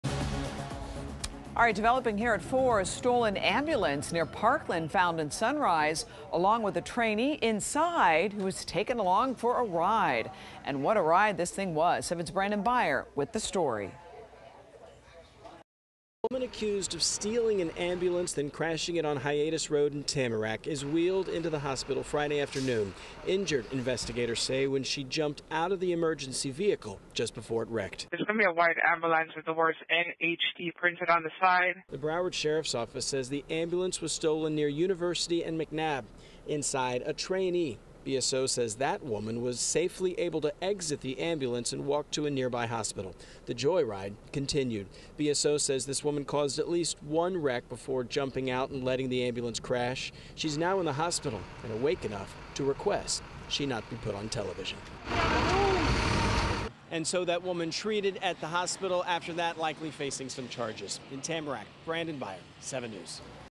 Grand Theft Ambulance: Suspect’s bumpy ride ends in crashes and cuffs in Broward County
A private ambulance was stolen on Friday afternoon, leading to a series of crashes in Broward County.